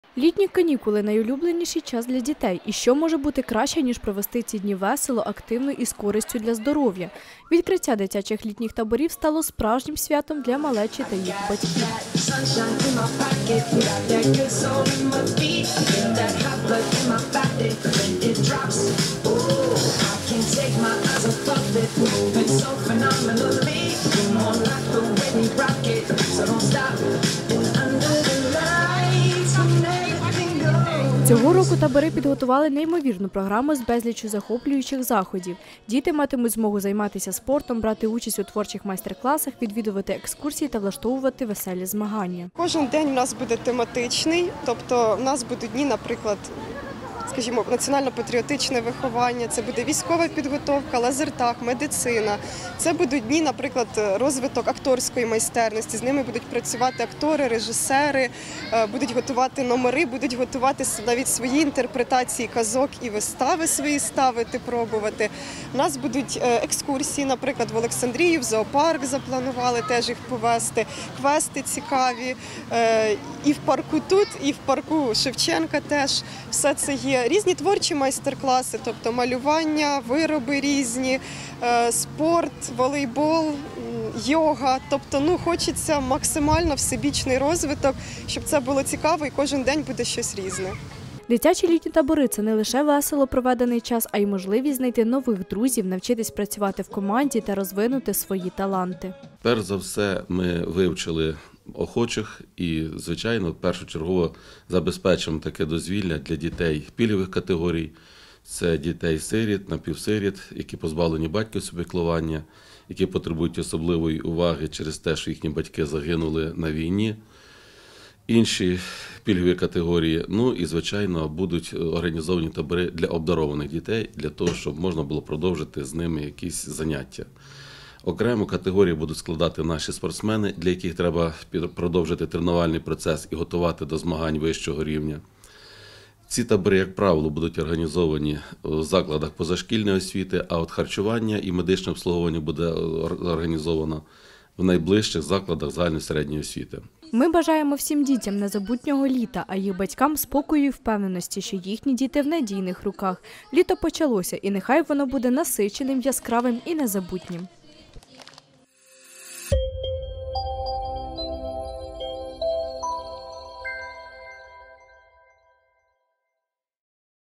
Святкове відкриття дитячих літніх таборів у Білій Церкві
Відкриття дитячих літніх таборів у Білій Церкві  стало справжнім святом для малечі та їх батьків. Цього року табори підготували насичену програму з безліччю захоплюючих заходів, що обіцяють веселі, активні та корисні дні.